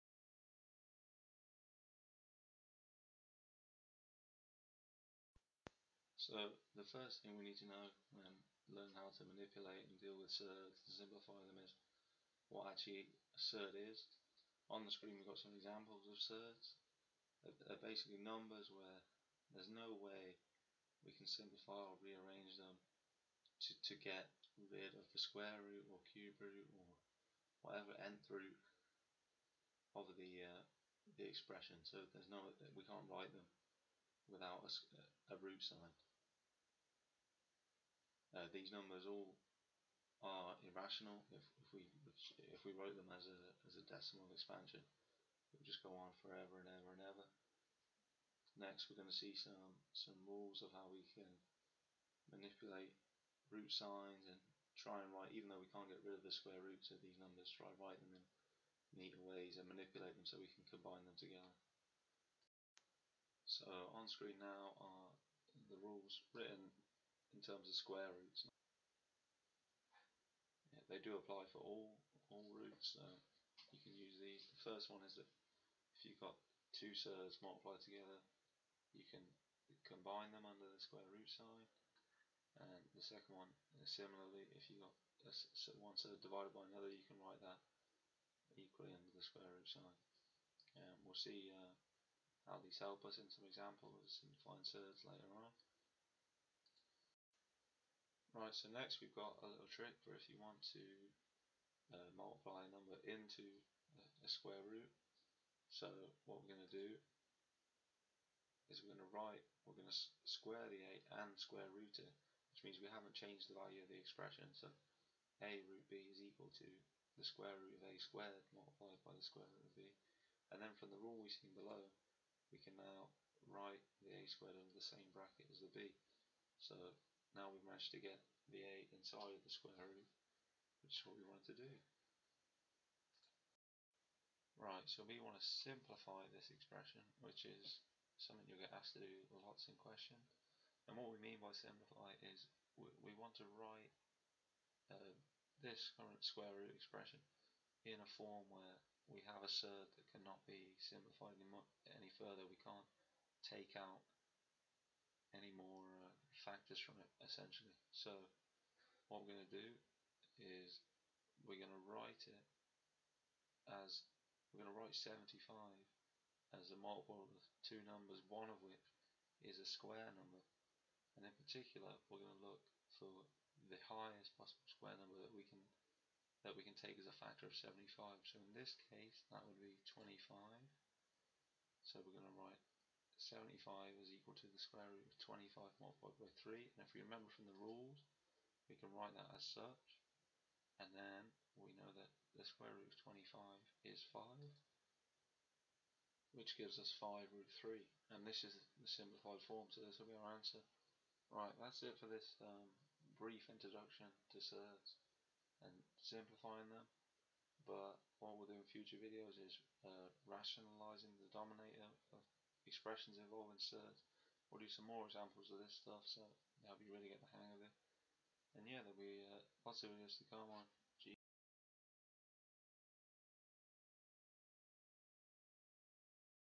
Intro to surds| What is a surd? Simplifying and rules
This video introduces surds and explains how to simplify them. A video in future will show how to rationalise fractions involving surds.